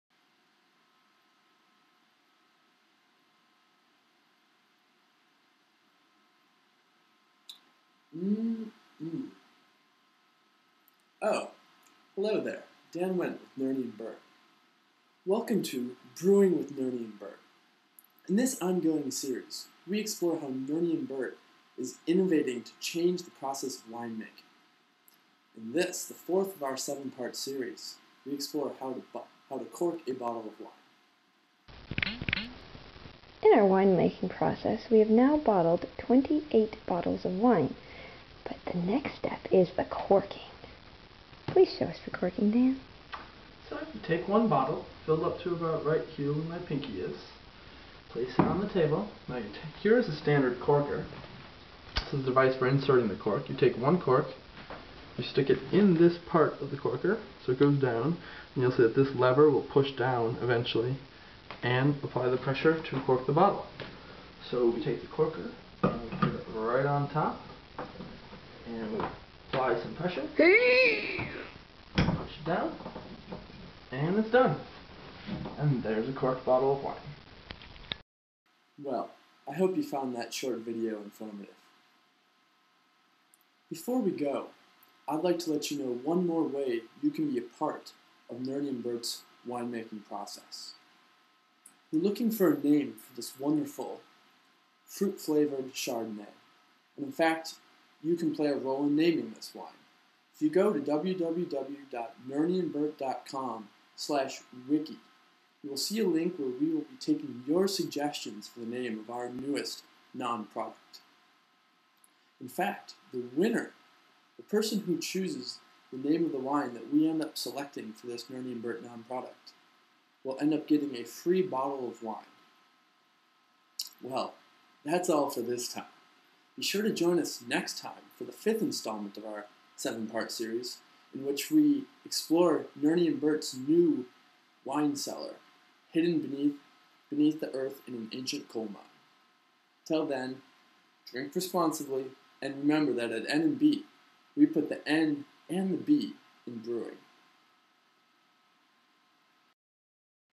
Brewing with Nernie & Bert (Part 4)
The 4th part in our 7 part series: Brewing with Nernie & Bert.  This vlog covers how to cork a bottle of wine, and announces a contest to name N&B's newest wine.